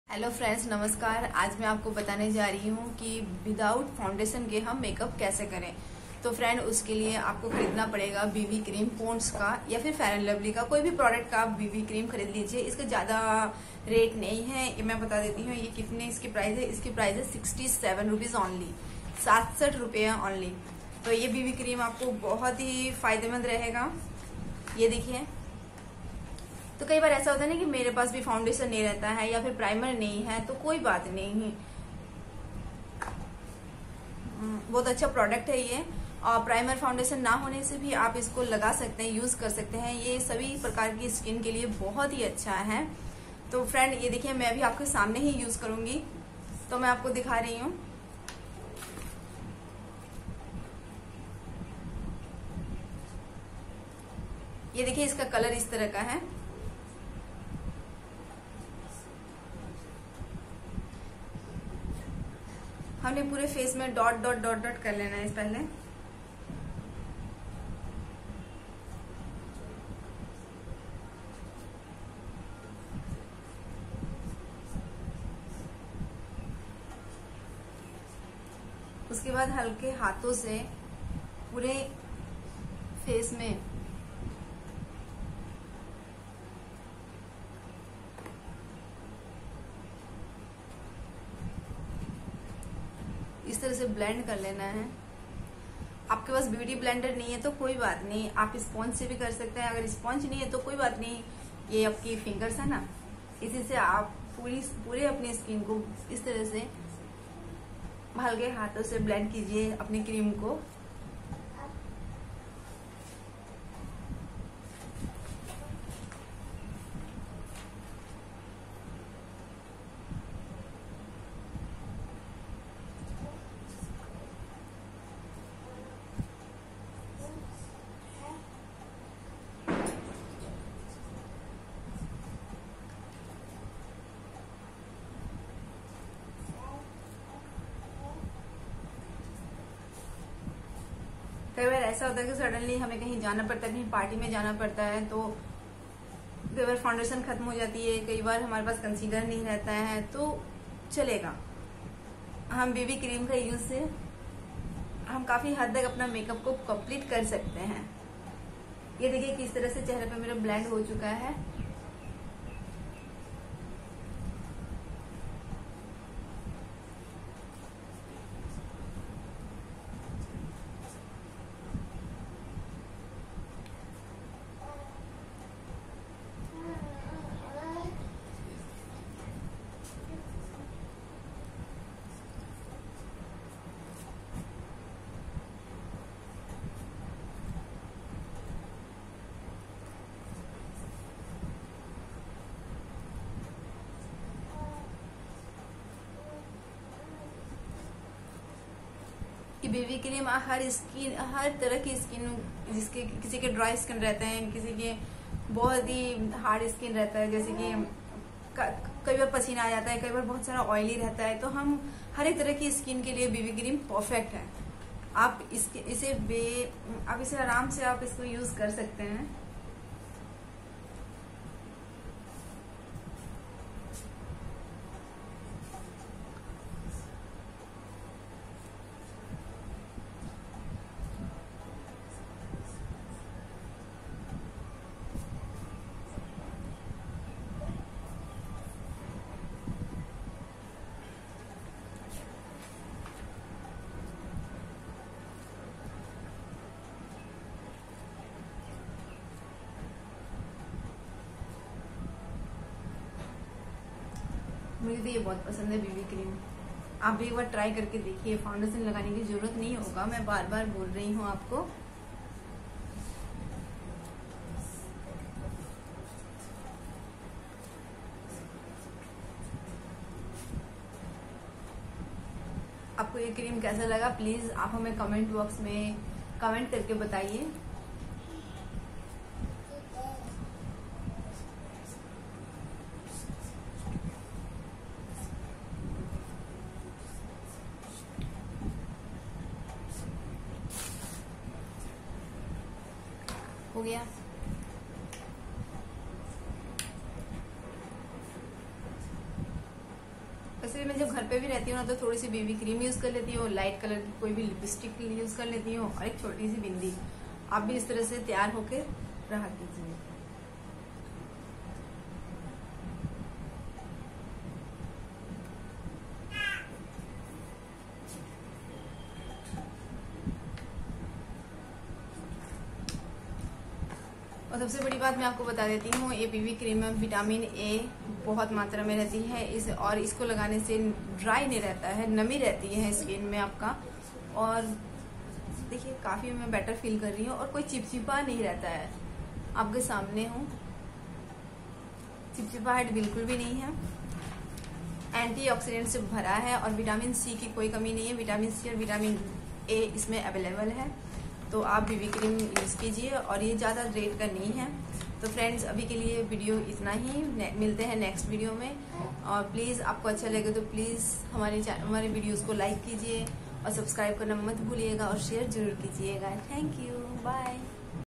जानिए BB Cream के बारे में🥰How to use BB cream// No Foundation //no Concealer// no primer
How to use BB cream,, सिर्फ BB Cream से आपना मेकअप कैसे करें 🥰🥰